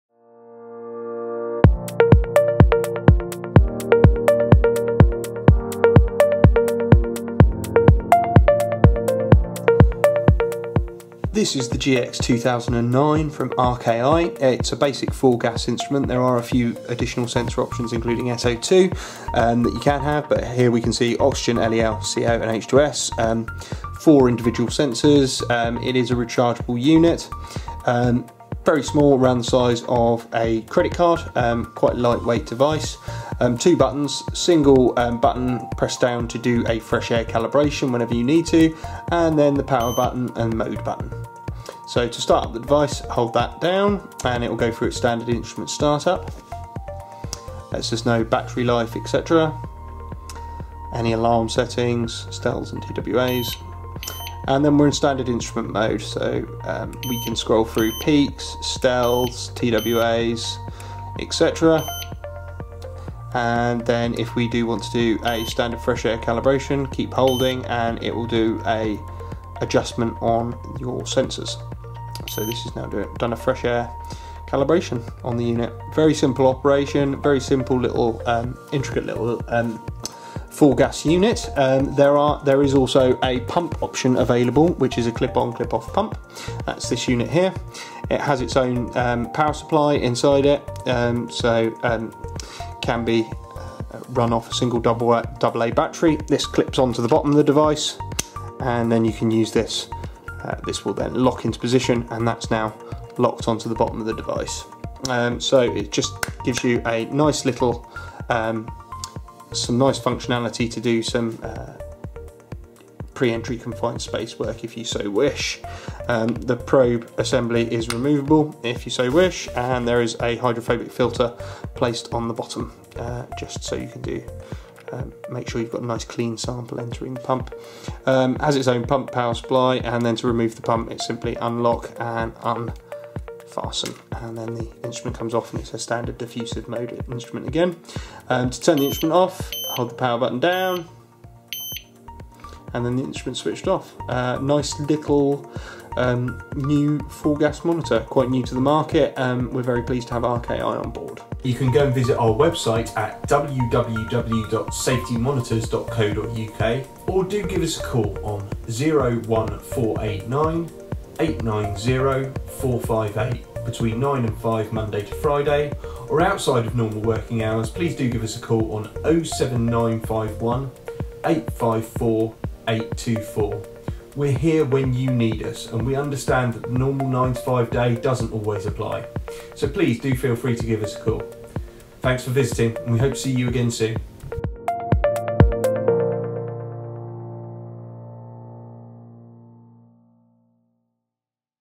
RKI GX 2009 4 Gas Monitor User Guide
This provides a basic user guide for the RKI GX 2009 4 Gas Monitor